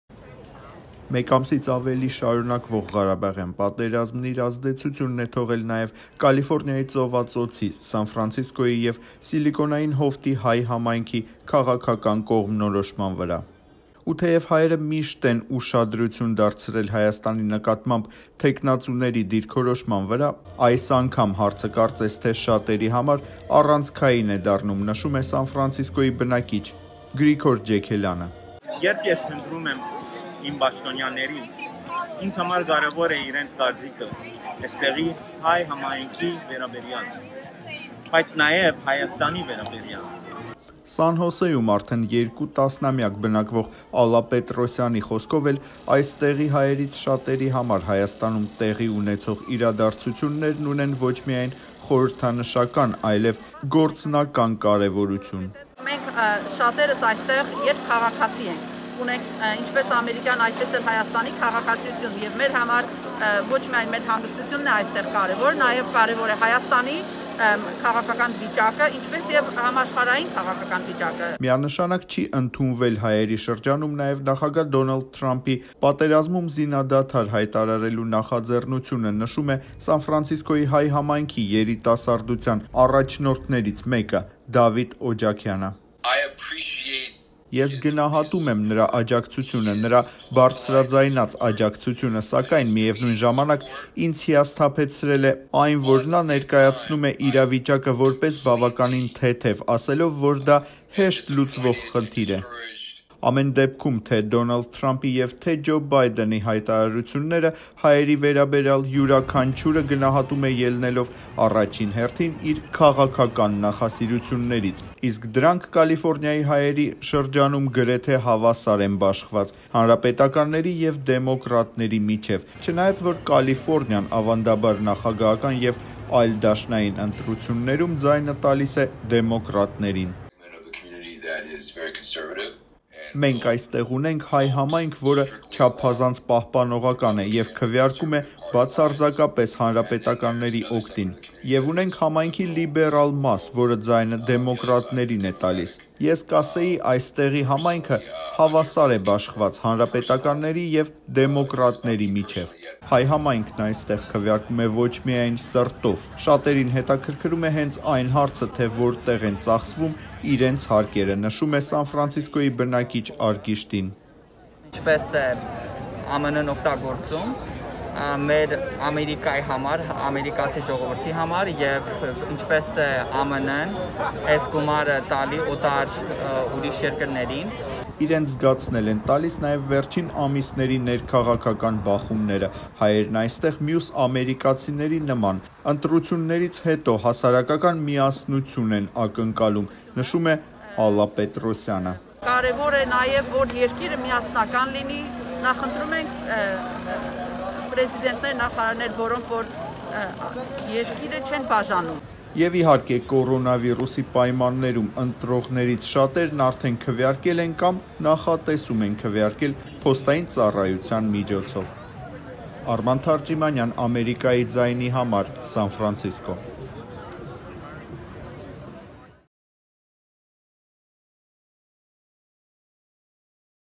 Ինչպե՞ս են քվեարկելու Կալիֆոռնիայի Ծովածոցի հայերը ԱՄՆ-ի առաջիկա ընտրություններում
Տեղի հայ համայնքը գրեթե հավասարապես կիսված է հանրապետական եւ դեմոկրատ թեկնածուների միջեւ: